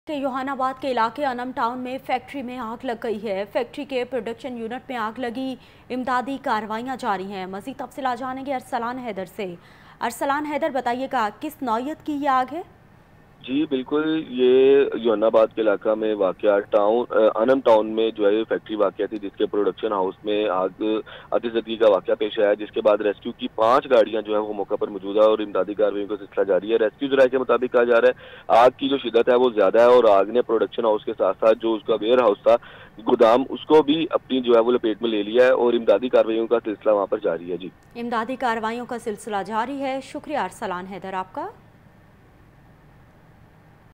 Fire breaks out in factory situated in Youhanabad, Lahore
Fire breaks out in factory situated in Youhanabad.